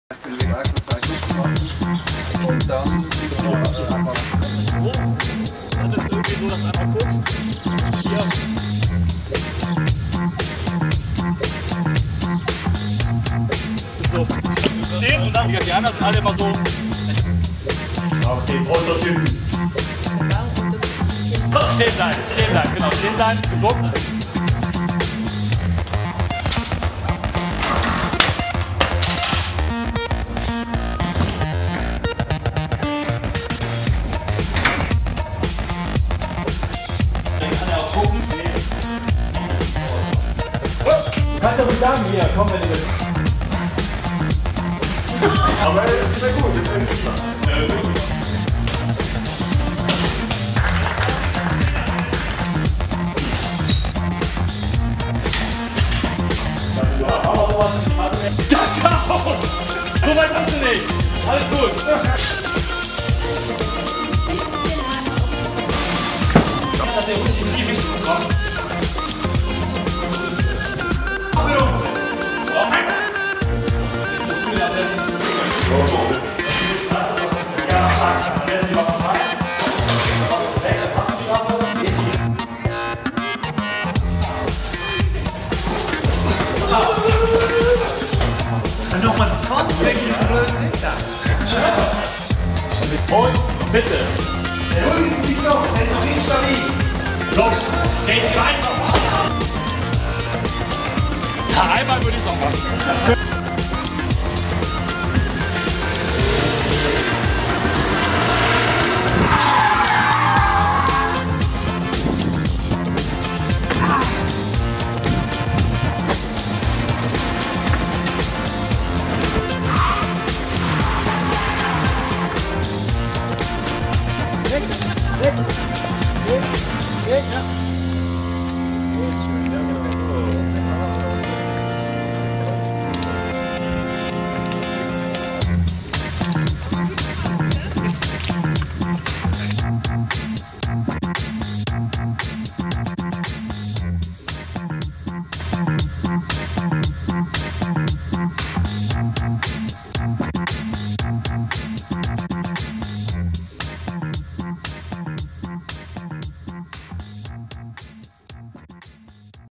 Making of Actionfilm als Firmen-Event
Firmen-Event der Extra-Klasse: Produzieren Sie Ihren eigenen Actionfilm. Stuntman Holger Schumacher und das Produktionsteam von Komet Film machen das möglich. Hier geht's zum Film